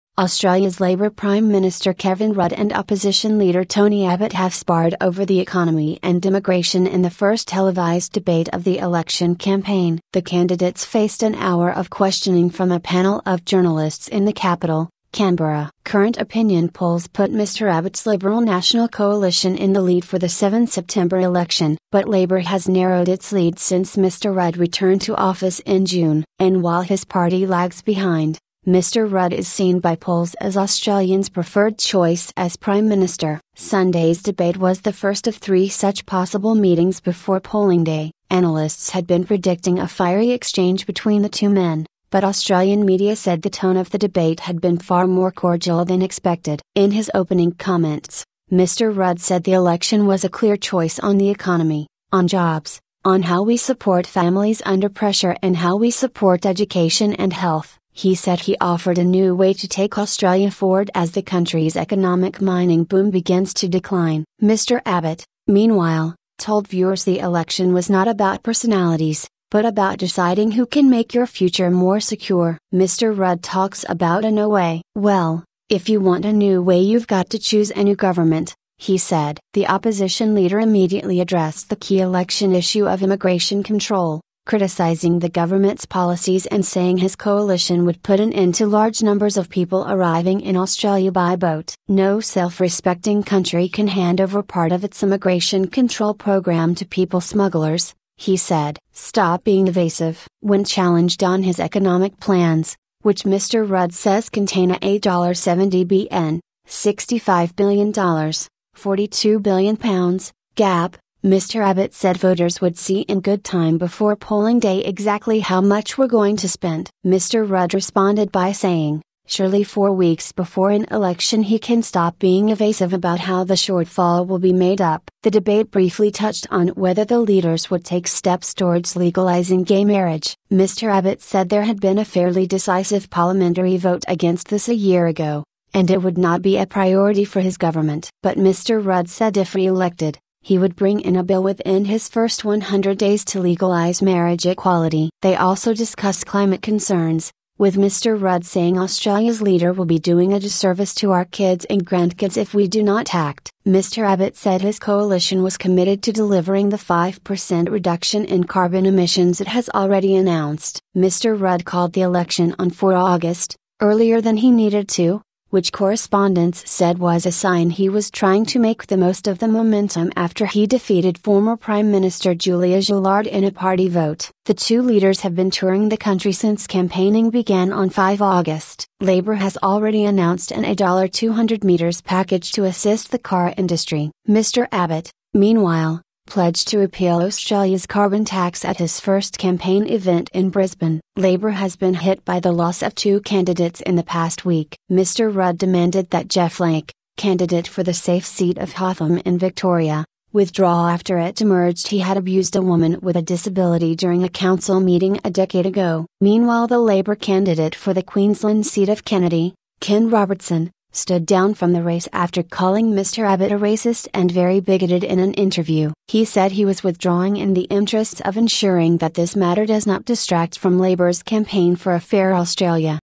Australian Candidates Face Off In Election Debate
Australia's Labor Prime Minister Kevin Rudd and opposition leader Tony Abbott have sparred over the economy and immigration in the first televised debate of the election campaign.

The candidates faced an hour of questioning from a panel of journalists in the capital, Canberra.

Current opinion polls put Mr Abbott's Liberal-National coalition in the lead for the 7 September election.

But Labor has narrowed its lead since Mr Rudd returned to office in June.

And while his party lags behind, Mr Rudd is seen by polls as Australians' preferred choice as prime minister.

Sunday's debate was the first of three such possible meetings before polling day.

Analysts had been predicting a fiery exchange between the two men, but Australian media said the tone of the debate had been far more cordial than expected.

In his opening comments, Mr Rudd said the election was "a clear choice on the economy, on jobs, on how we support families under pressure and how we support education and health".

He said he offered a "new way to take Australia forward" as the country's economic mining boom begins to decline.

Mr Abbott, meanwhile, told viewers the election was not about personalities, but about deciding "who can make your future more secure".

"Mr Rudd talks about a new way. Well, if you want a new way you've got to choose a new government," he said.

The opposition leader immediately addressed the key election issue of immigration control, criticising the government's policies and saying his coalition would put an end to large numbers of people arriving in Australia by boat.

"No self-respecting country can hand over part of its immigration control programme to people smugglers," he said.
'Stop being evasive'

When challenged on his economic plans - which Mr Rudd says contain a A$70bn ($65bn: £42bn) gap - Mr Abbott said voters would "see in good time before polling day exactly how much we're going to spend".

Mr Rudd responded by saying: "Surely four weeks before an election he can stop being evasive" about how the shortfall will be made up.

The debate briefly touched on whether the leaders would take steps towards legalising gay marriage.

Mr Abbot said there had been a "fairly decisive" parliamentary vote against this a year ago, and it would not be a priority for his government.

But Mr Rudd said if re-elected, he would bring in a bill within his first 100 days to legalise marriage equality.

They also discussed climate concerns, with Mr Rudd saying Australia's leader "will be doing a disservice to our kids and grandkids if we do not act".

Mr Abbott said his coalition was committed to delivering the 5% reduction in carbon emissions it has already announced.

Mr Rudd called the election on 4 August, earlier than he needed to, which correspondents said was a sign he was trying to make the most of the momentum after he defeated former prime minister Julia Gillard in a party vote.

The two leaders have been touring the country since campaigning began on 5 August.

Labor has already announced an A$200m package to assist the car industry.

Mr Abbott, meanwhile, pledged to repeal Australia's carbon tax at his first campaign event in Brisbane.

Labor has been hit by the loss of two candidates in the past week. Mr Rudd demanded that Geoff Lake, candidate for the safe seat of Hotham in Victoria, withdraw after it emerged he had abused a woman with a disability during a council meeting a decade ago.

Meanwhile the Labor candidate for the Queensland seat of Kennedy, Ken Robertson, stood down from the race after calling Mr Abbott a racist and "very bigoted" in an interview.

He said he was withdrawing "in the interests of ensuring that this matter does not distract from Labor's campaign for a fairer Australia".